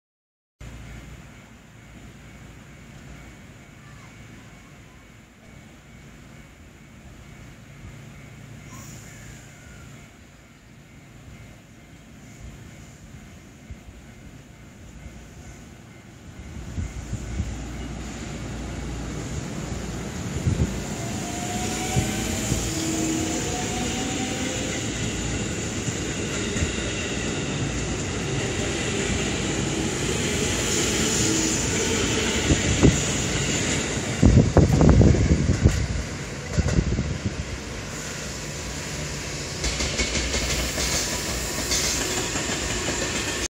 【207系唯一の7両貫通車】JR西日本207系0番台F1編成 普通西明石行 東淀川発車！
207系唯一の7両貫通車F1編成です。普通の207系は4両編成と3両編成を常に連結させて計7両編成で運行していますが、このF1編成は207系の量産先行車で7両で1編成扱いとなっています。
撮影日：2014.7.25